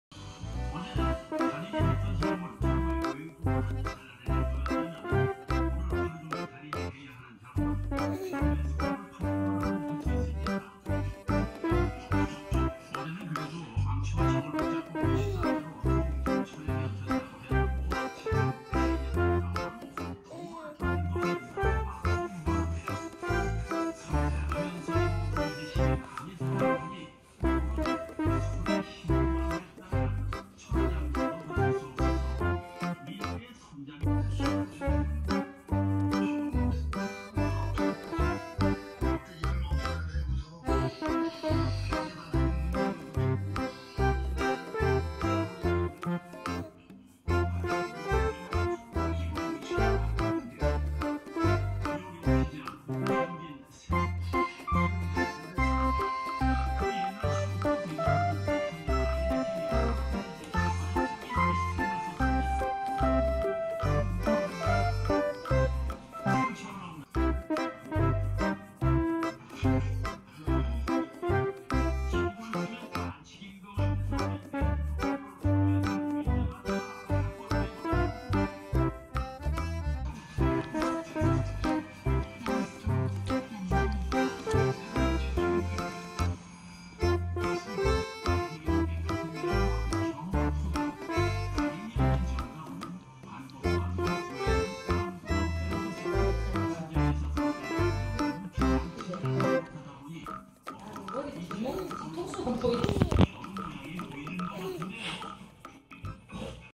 아기고양이와 동거3일차
2일차와 3일차의 변화가 엄청났습니다
몸의 움직임이 민첩해지고
한번에 분유를 빠는 양도 3배정도 늘었습니다